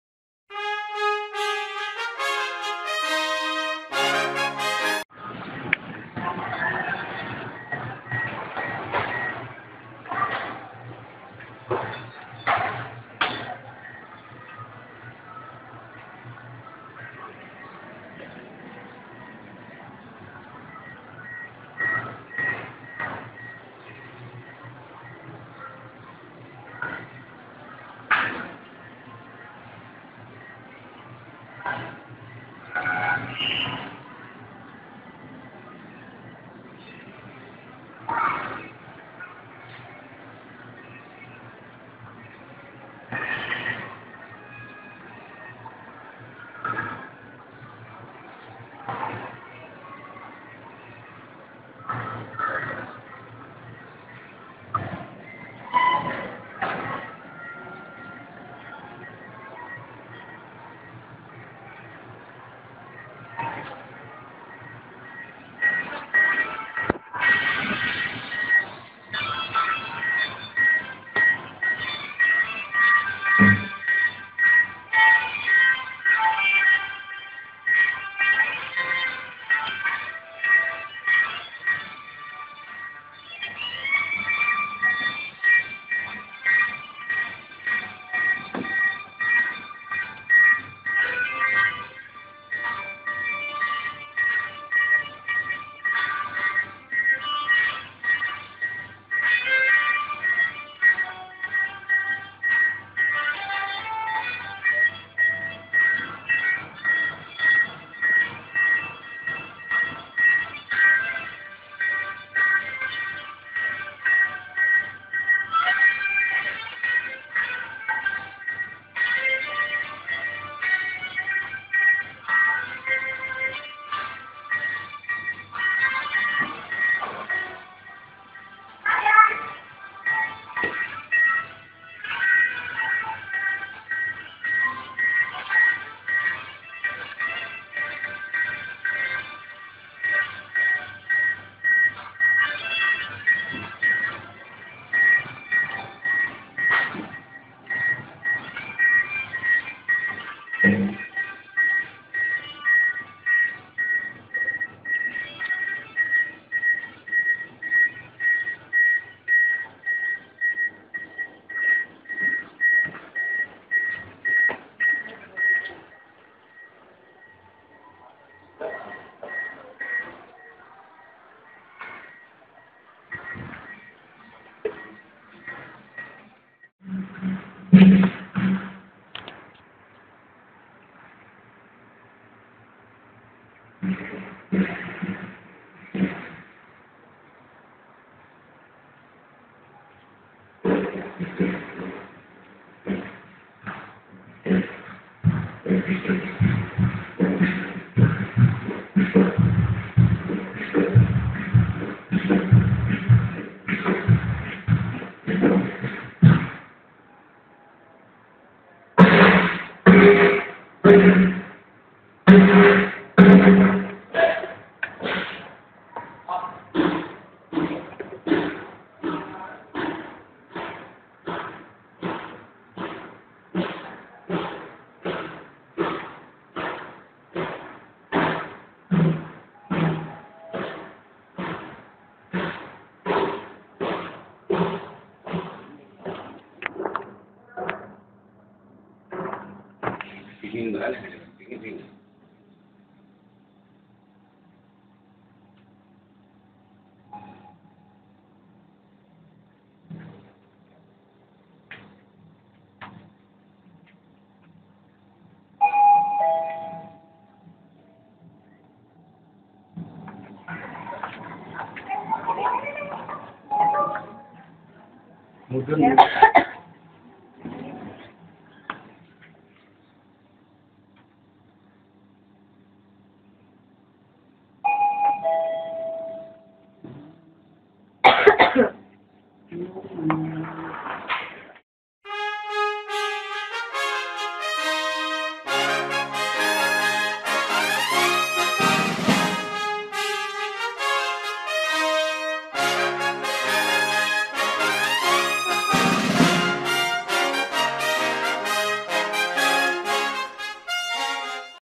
[REMASTERED FIRST TAKES] 2x Sigma Lifts / Elevators - Plaza Semanggi + Gedung Veteran RI, Jakarta
My first takes of a Sigma Di1 and Sigma Di2 here

0:00 - Plaza Semanggi parking lift
2:56 - Gedung Veteran RI lift
Original date of recording: February 25th, 2012